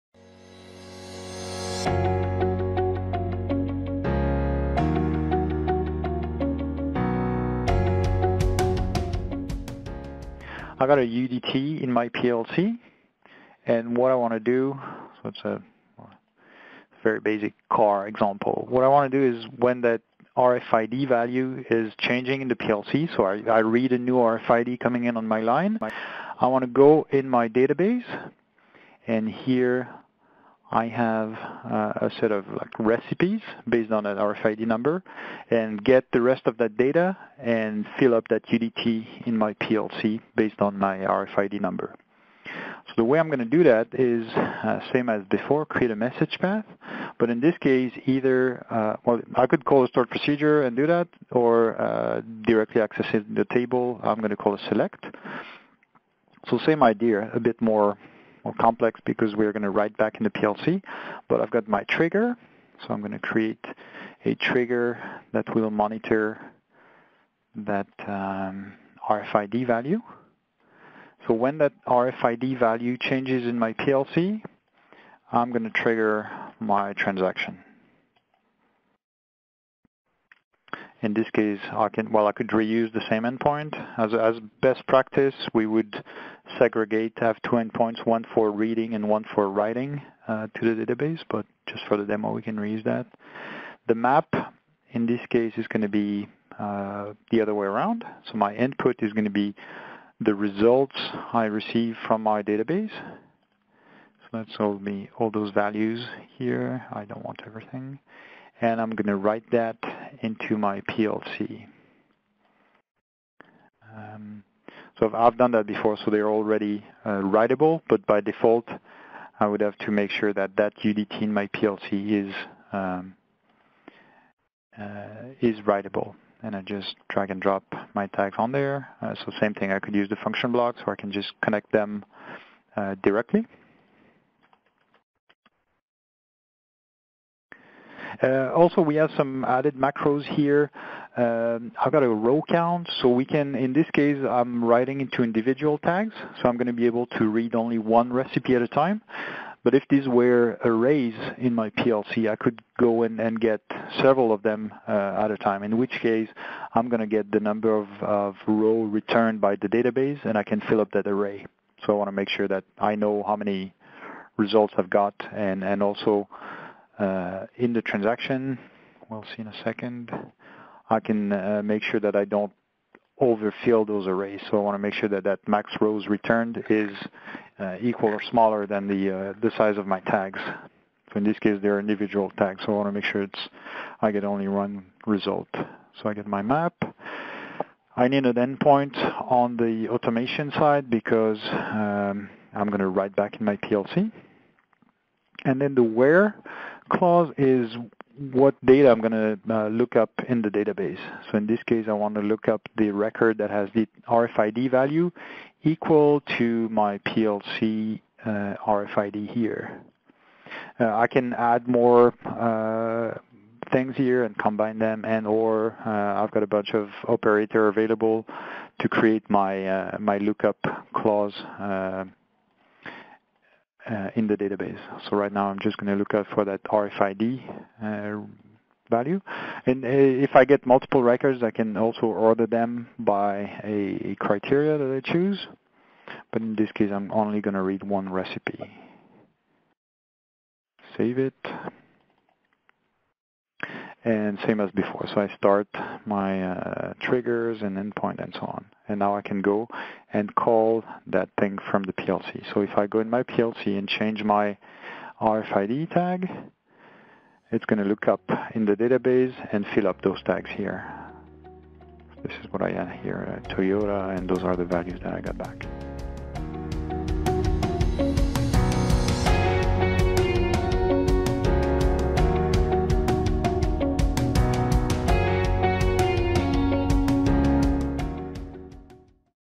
tManager Training. Tutorial 2 of 2. Read tags from SQL Database. Write to PLC.
The complete getting started guide for Softing's tManager for downloading a recipe from an SQL database to a ControlLogix or CompactLogix PLC.